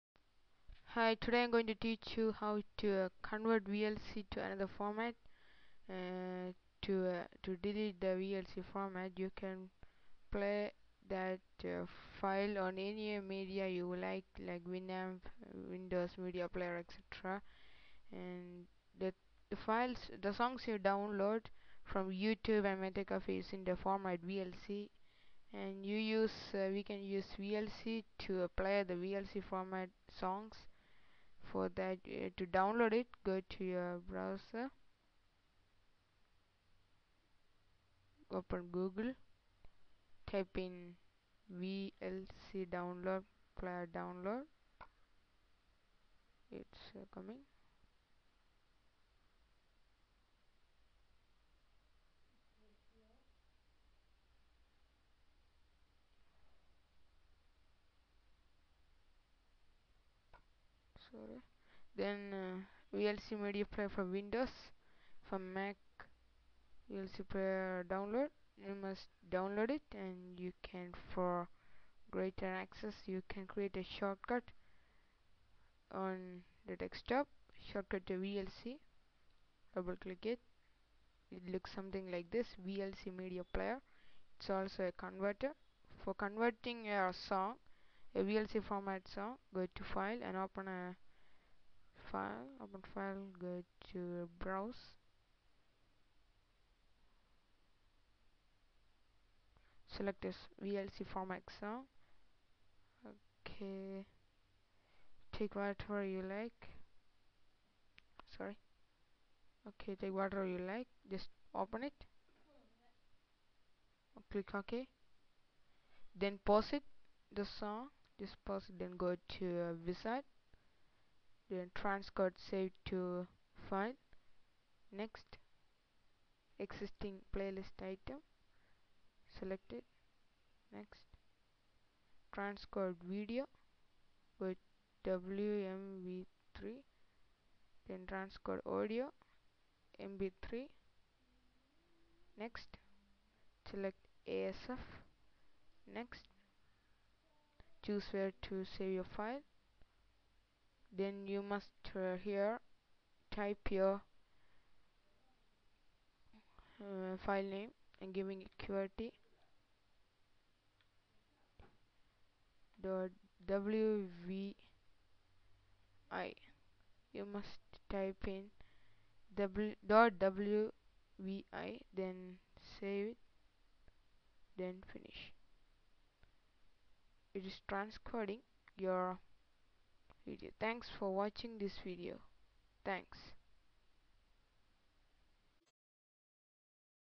how to change vlc format
i am going to teach you how to change vlc format using vlc player! (for more tips and tricks go